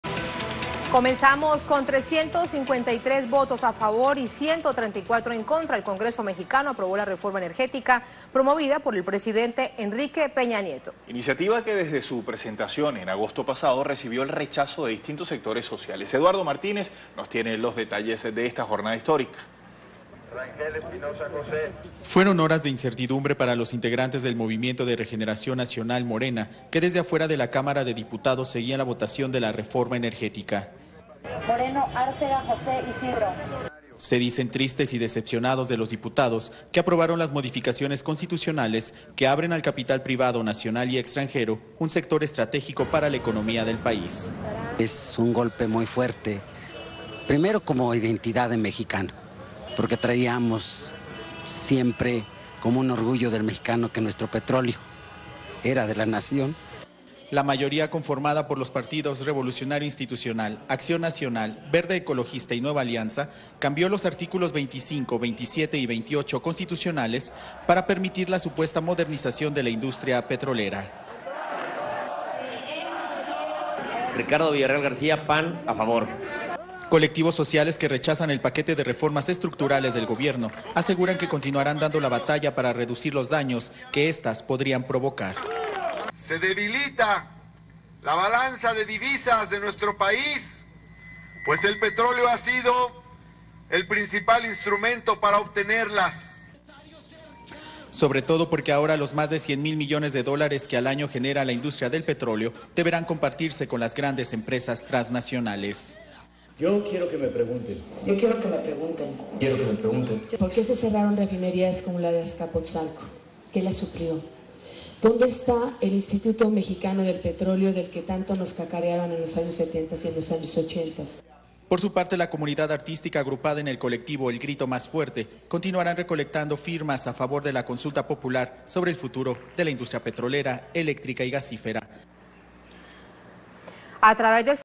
Energy reform approved by Chamber of Deputies in Mexico
The Mexican Congress approved the energy reform promoted by Enrique Peña Nieto's government with 353 votes in favour and 134 against.  The initiative, which was revealed in August, is rejected by many social sectors. teleSUR